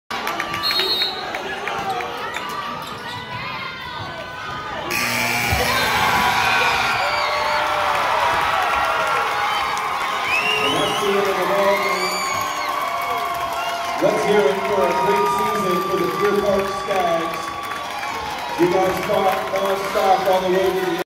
Final play - Deer Park vs. Elma - 1A Girls Basketball Regional
The final play of Elma's 39-38 victory in the 1A girls basketball Regional on Saturday, Feb. 29, 2020 at Tumwater High School.